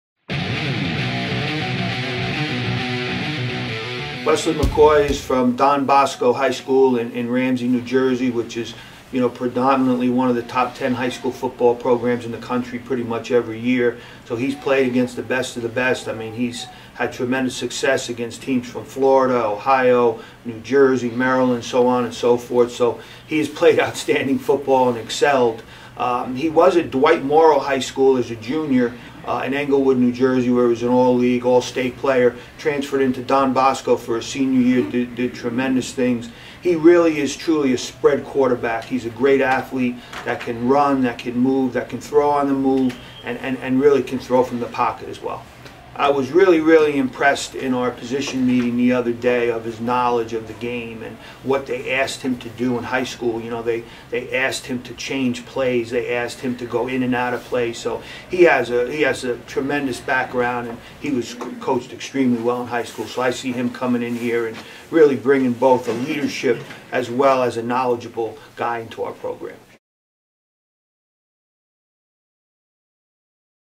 2015 Recruit - Wesley McKoy
A dynamic weapon for the powerful Don Bosco program, Wesley McCoy was an All-League, All-County and All-Metro pick as a senior. Capable of making plays with his arms and his legs, McCoy accounted for 36 touchdowns over his final two seasons, throwing 24 and running for 12 more. He rushed for more than 1,500 yards and passed for more than 2,500. McCoy led Don Bosco to a 24-7 upset of national power St. Thomas Aquinas last September.